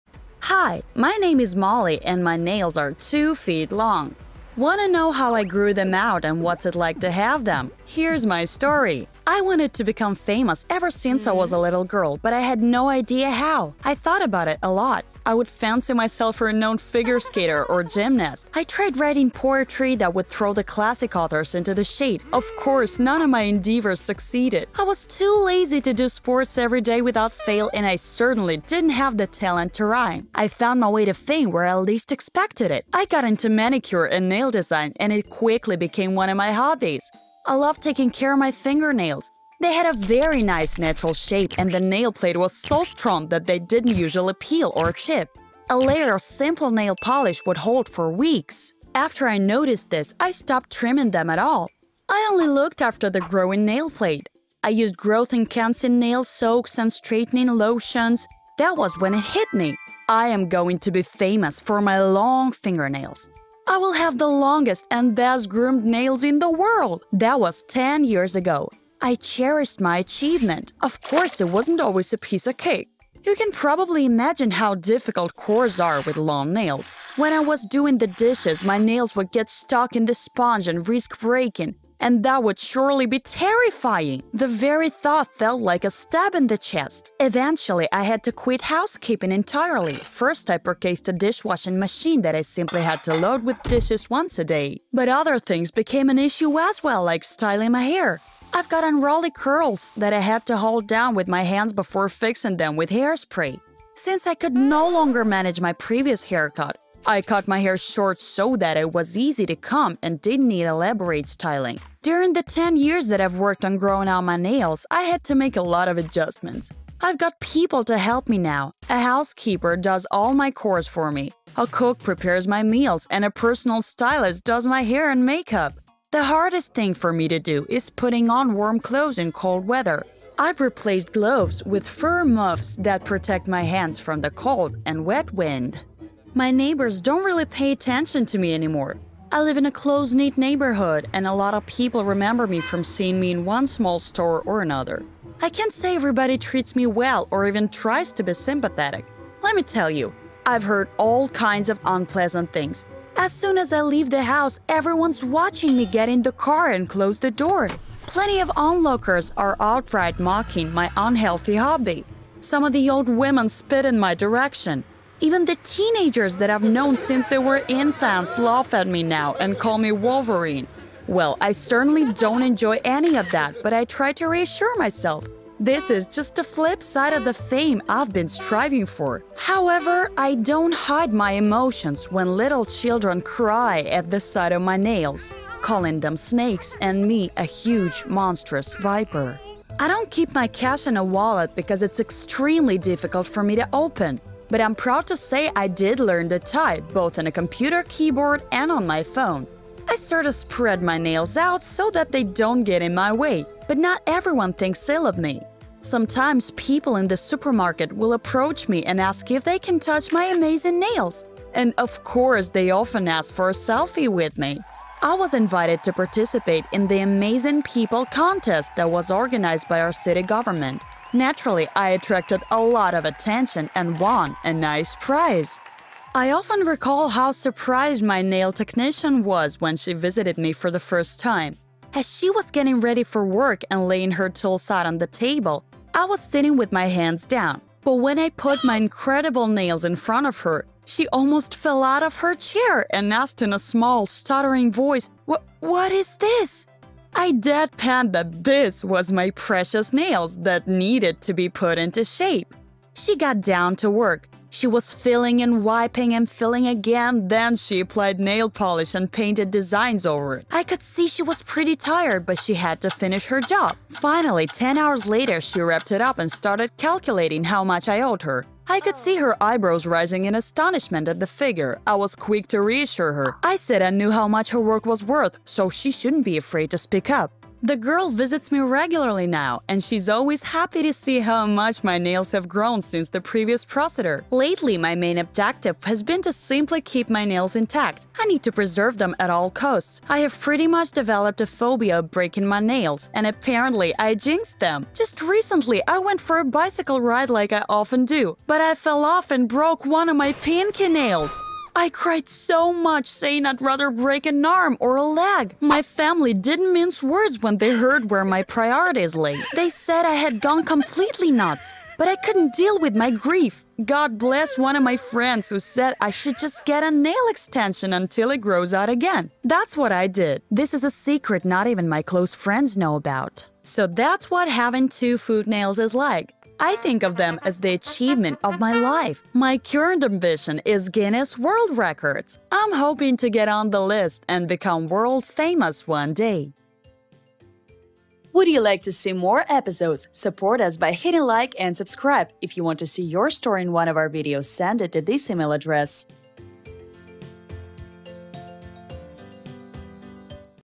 I Grew Two-foot Nails And Here's What It's Like To Live With Them
Molly's nails are two feet long. Her story is about how she grew them out and what it's like to have them.

Actually happened Molly wanted to become famous ever since she was a little girl but she had no idea how. She'd fancy herself a renowned figure skater or gymnast. She tried writing poetry that would throw the classic authors into the shade. Of course, none of her endeavors succeeded. She was too lazy to do sports every day without fail, and she certainly didn't have the talent to rhyme.

She found her way to fame where she least expected it, when manicure and nail design became one of her hobbies. Molly loved taking care of her fingernails. They had a very nice natural shape and the nail plate was so strong that they didn't usually peel or chip. A layer of simple nail polish would hold for weeks.

After Molly noticed this, she stopped trimming them at all. She used growth enhancing nail soaks and strengthening lotions. And it hit her: she's going to have the longest and best-groomed nails in the world. That was 10 years ago.

It wasn't always a piece of cake. When Molly was doing the dishes, her nails would get stuck in the sponge and risk breaking. And that would surely be terrifying! The very thought felt like a stab in the chest. Eventually, Molly had to quit housekeeping entirely. First, she purchased a dishwashing machine. After she could no longer manage her hair, she cut it short so that it was easy to comb and didn't need elaborate styling. She's got people to help her now. A housekeeper does all her chores, a cook prepares her meals, and a personal stylist does her hair and make-up. The hardest thing for Molly to do is putting on warm clothes, so she's replaced gloves with fur muffs. She doesn't keep her cash in a wallet because it's extremely difficult for her to open. But she did learn to type on a keyboard and on her phone. She spreads her nails out so that they don't get in her way.

Not all of Molly's neighbors are sympathetic. As soon as she leaves the house, everyone's watching her get in the car and close the door. Plenty of onlookers are outright mocking her unhealthy hobby. Some of the old women spit in her direction. Even the teenagers that she's known since they were infants laugh at her now and call her Wolverine. Little children cry at the sight of her nails. Molly certainly doesn't enjoy any of that but she tries to reassure herself: this is just the flip side of the fame she's been striving for.

But not everyone thinks ill of her. Sometimes, people in the supermarket will approach Molly and ask if they can touch her amazing nails or take a selfie with her. She was invited to participate in the Amazing People Contest that was organized by her city government. Naturally, she attracted a lot of attention and won a nice prize.

Molly often recalls how surprised her nail technician was when she visited her for the first time. As the girl was laying her tools out on the table, Molly was sitting with her hands down. But when Molly put her incredible nails in front of her, she almost fell out of her chair and asked in a small, stuttering voice: "W-what is this?" Molly deadpanned that "this" was her precious nails that needed to be put into shape. The girl got down to work. She was filing and wiping and filing again. Then she applied nail polish and painted designs over it. Molly could see she was pretty tired but she had to finish her job.

Finally, 10 hours later, she wrapped it up and started calculating how much Molly owed her. Her eyebrows rose in astonishment at the figure but Molly was quick to reassure her, saying she knew how much her work was worth.

This girl visits Molly regularly now and she's always happy to see how much her nails have grown since the previous procedure.

Lately, Molly's main objective has been to simply keep her nails intact. She needs to preserve them at all costs. She's pretty much developed a phobia of breaking her nails. And apparently she jinxed them! Just recently, she fell off her bicycle and broke one of her pinkie nails! She cried so much, saying she'd rather break an arm or a leg. Her family replied that she'd gone completely nuts. But Molly couldn't deal with her grief. Thankfully, one of her friends advised her to get a nail extension until it grows out again. This is Molly's secret now.

Molly thinks of her nails as the achievement of her life. Her current ambition is Guinness World Records. She's hoping to get on the list and become world-famous one day!